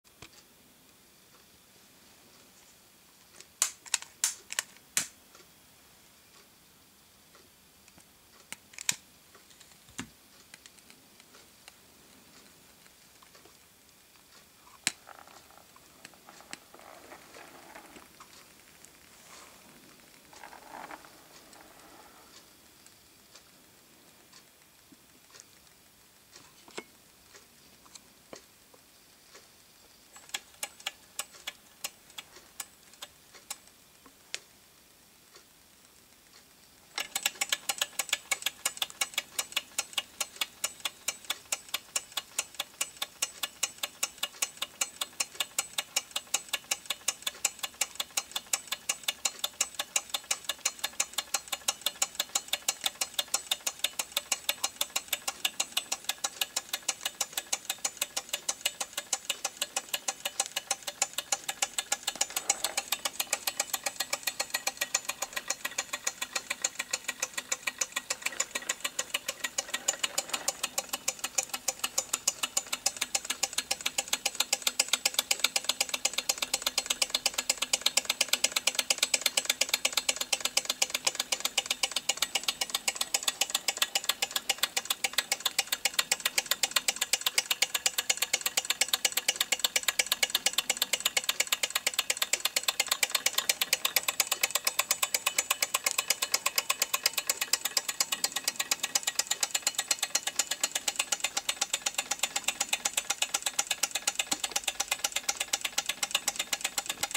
Stirling engine Reichelt , model LH
R. Reichelt Stirling engine Model ' LH' . The same model has been tested and run continuously from 3 sept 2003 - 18 dec 2005 , more than 20,000 hrs runtime!!